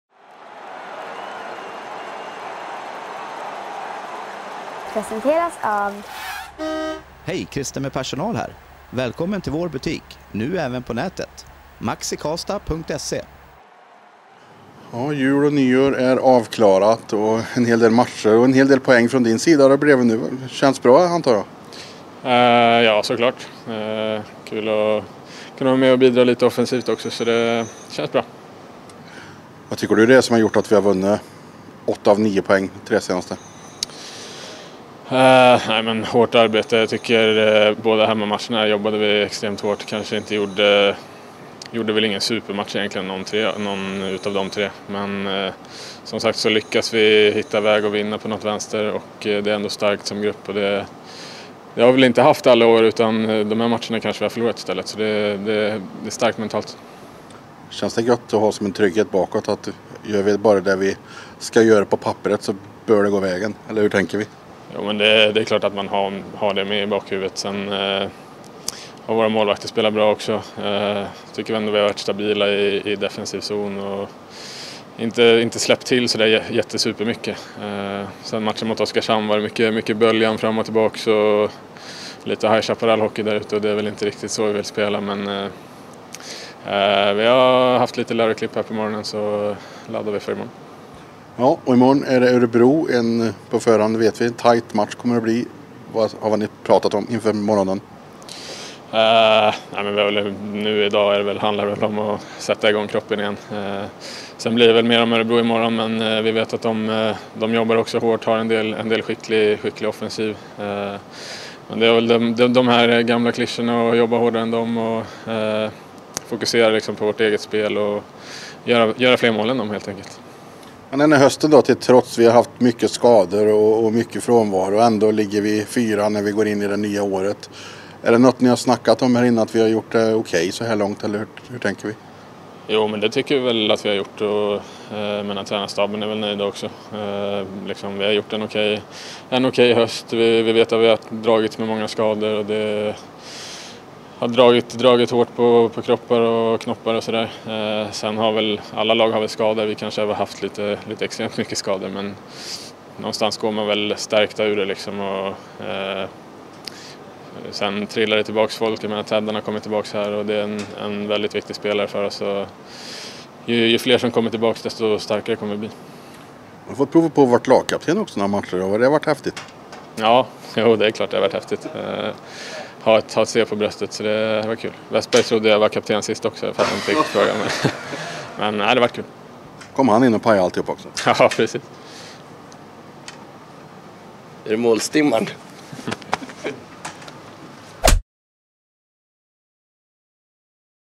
Årets första träning avklarad
Han lämnade 2022 i ett poängstim. Efter årets första träningspass tog vi ett snack med Joakim Nygård om de senaste matcherna och vår kommande match mot Örebro.